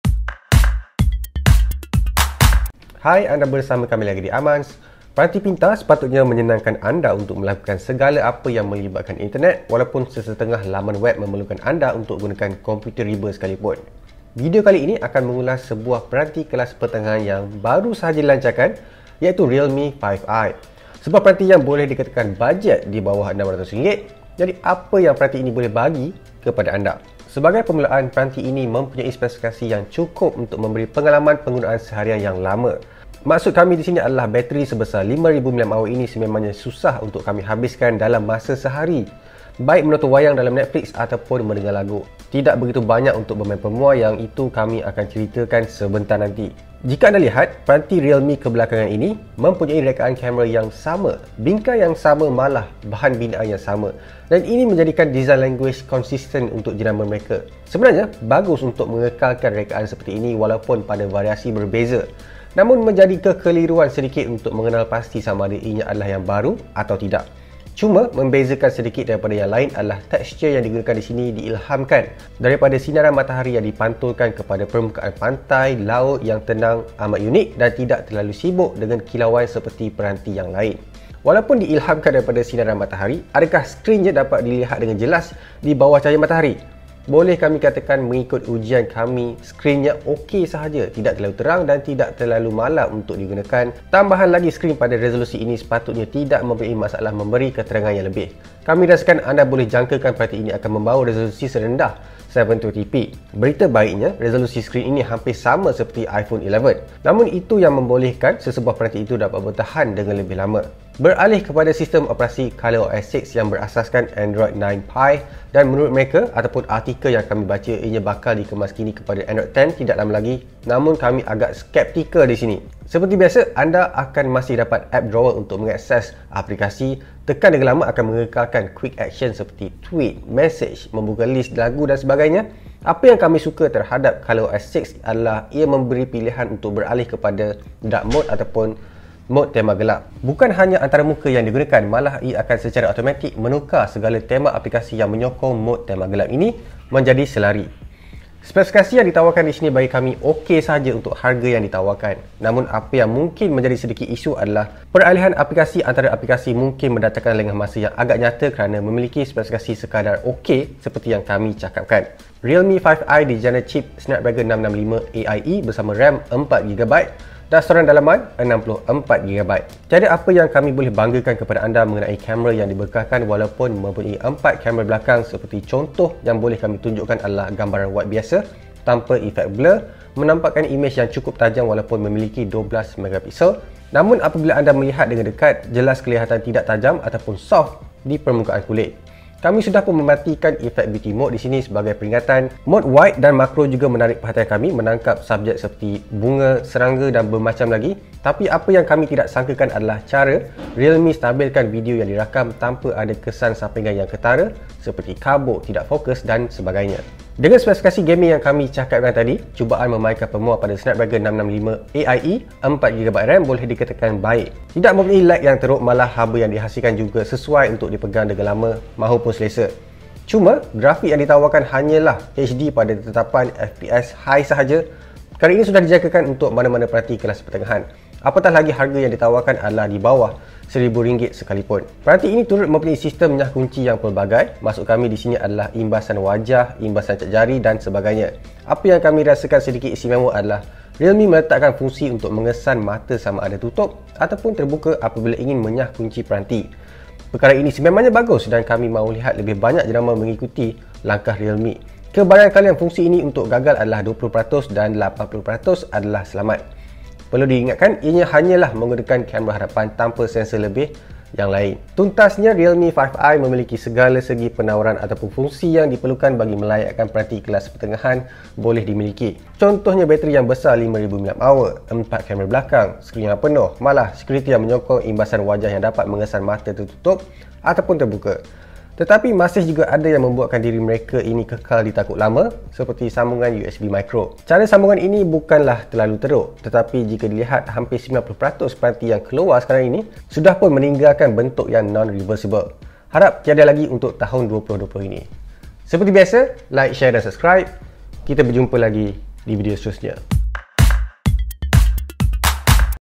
Realme 5i - Prestasi Dah Okay, Bagaimana Yang Lain? (Ulasan)
Realme 5i merupakan edisi terbaru realme tahun baru 2020 dan ia merupakan peranti kelas pertengahan mampu milik berharga RM599. Ianya dikuasakan oleh cip pemproses snapdragon 665, Ram sebesar 4Gb dan storan dalaman 64GB. Adakah peranti ini sesuai untuk anda yang mencari peranti lebih mampu milik? sila lihat video penuh.